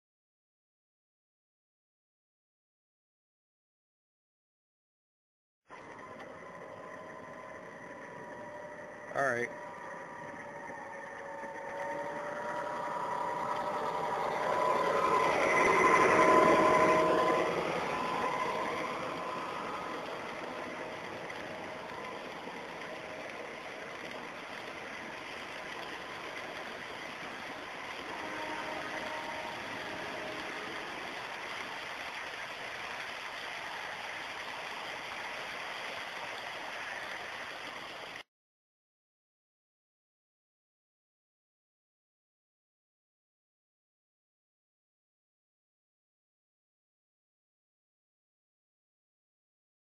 New SD70mac Pull Test
A little QUICKIE..Gene gets a new engine.. New USA Trains SD70.. No traction tires... More weight